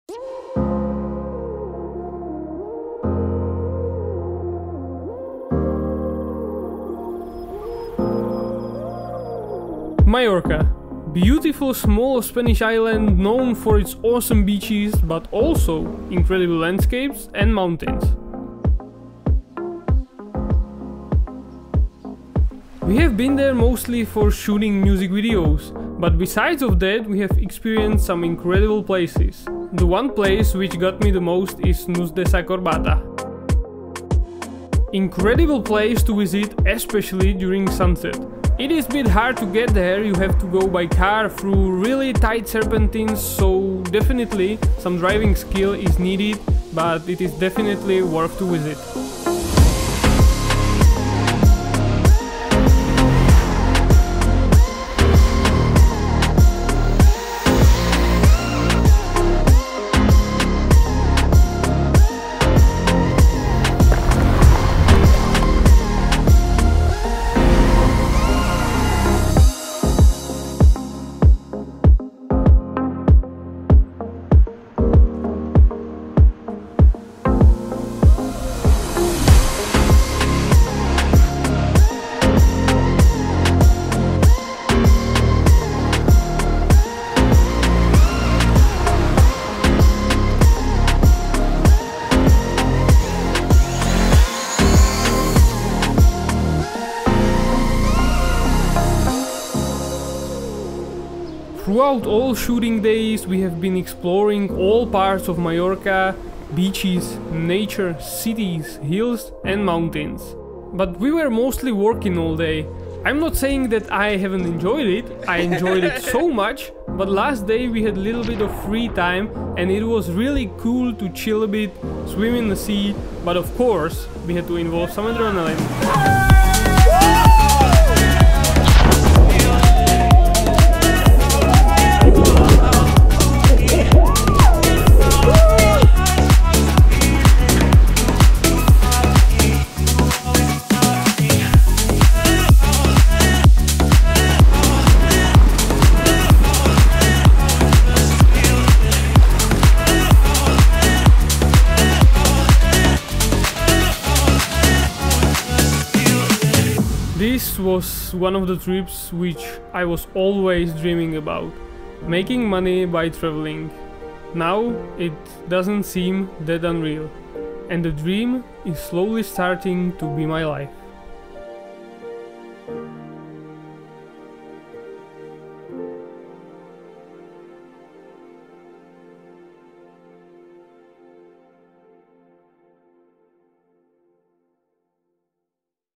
Why You Need to Visit MALLORCA | Cinematic travel video
We had the opportunity to film 4 music clips on Mallorca and also enjoy this travel trip. Mallorca is awesome small Spanish Island with incredible beaches but also landscapes, neture and mountains. Really great travel destination for some cinematic travel video. Also a few tips for epic places in this video. This was the point when I have realized dat making money by travelling is possible and it doesn't has to be just a dream. But it can be my life.

MALLORCA TEAM:
YoungHZ (artist):